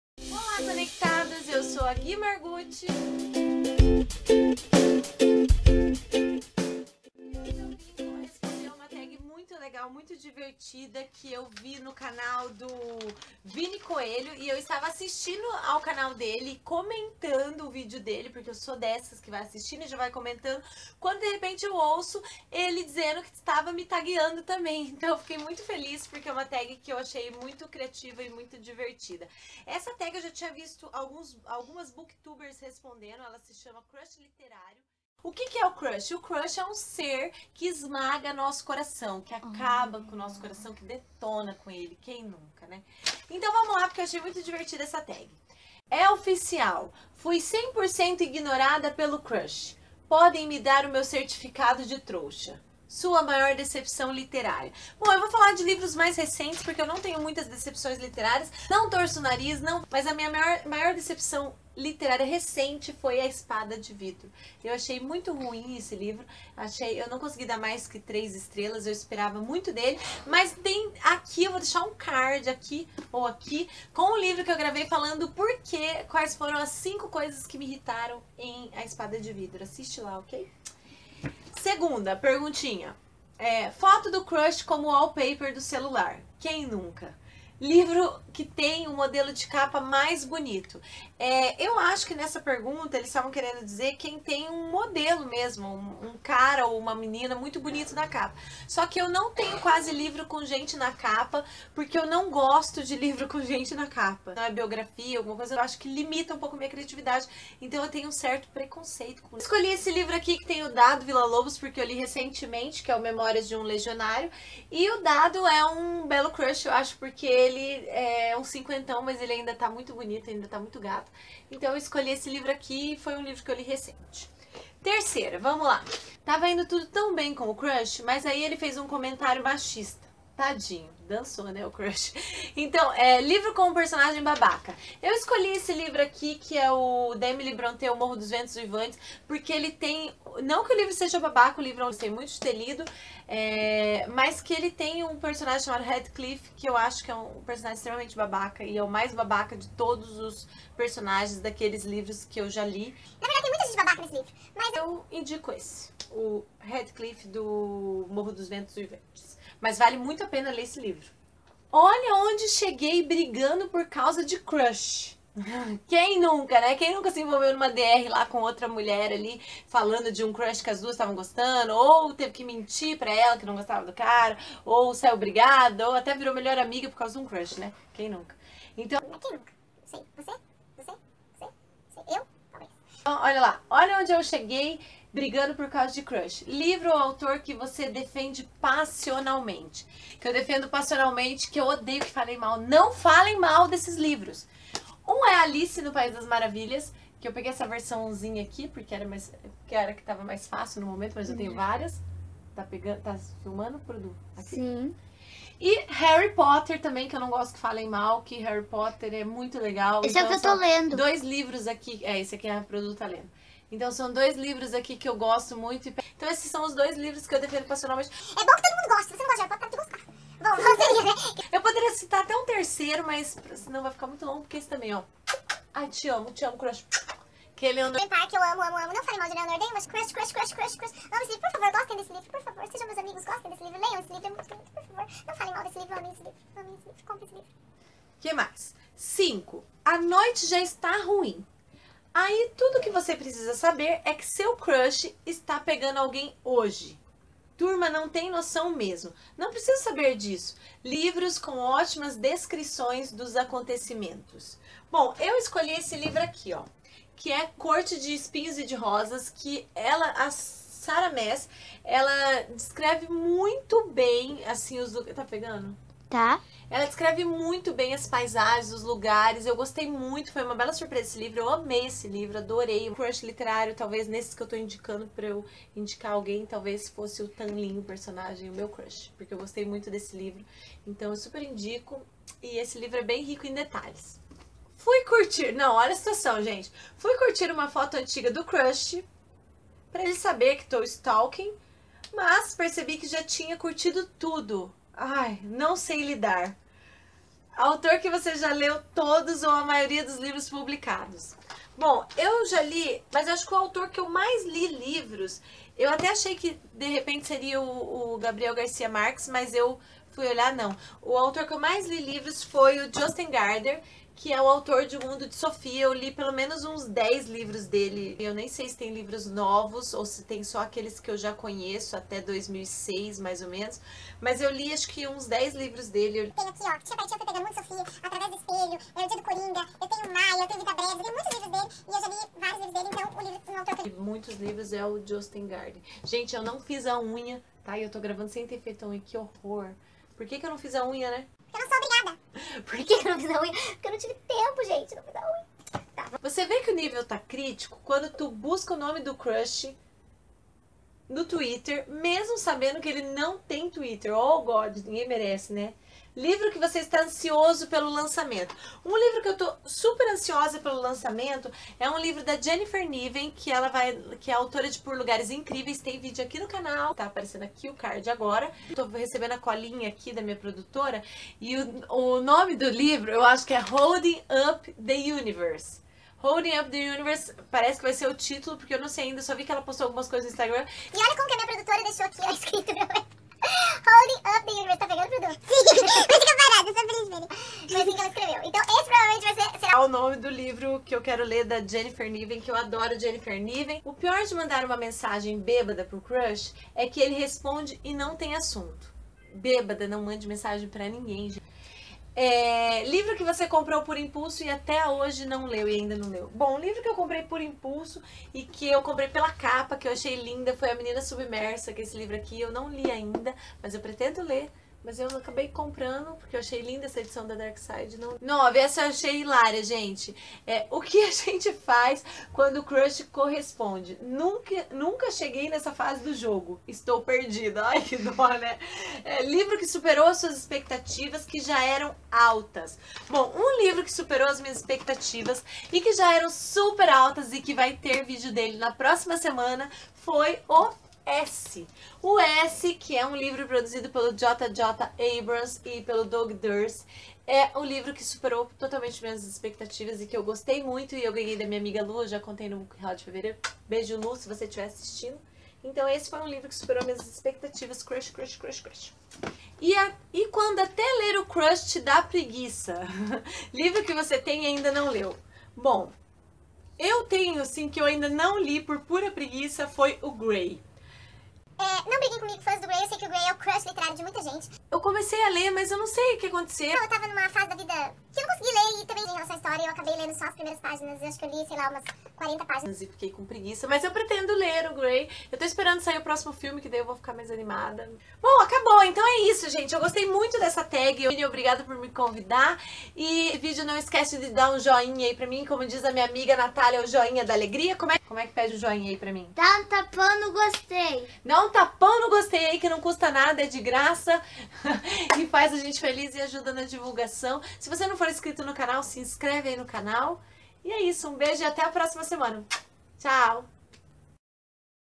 CRUSH LITERÁRIO - GUI MARGUTTI - COFFEEBOOKLOVER - TAG CRUSH LITERÁRIO
Olá conectados!  Eu fui convidada a responder a tag Crush Literária pelo Vini Coelho (link do canal abaixo). Adorei essa tag, achei divertidissima. Soube que o booktuber que a criou se inspirou no twitter e nas postagens sobre#crush.  Espero que gostem.

Beijos

Beijos

Beijos